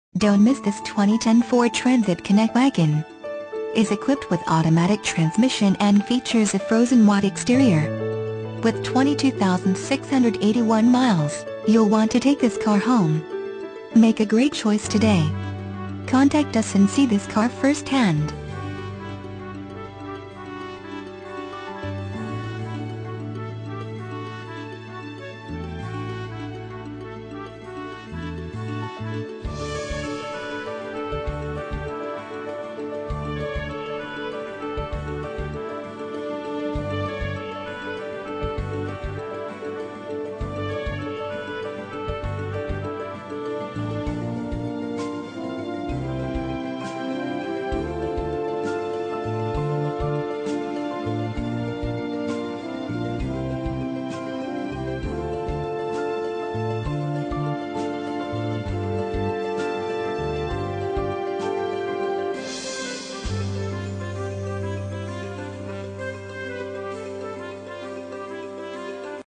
2010 FORD TRANSIT CONNECT WAGON Cleveland, GA U7156
2010 FORD TRANSIT CONNECT WAGON Cleveland, GA
Stock

Don't miss this 2010 Ford Transit Connect Wagon. It's equipped with automatic transmission and features a Frozen White exterior. With 22681 miles, you'll want to take this car home. Make a great choice today.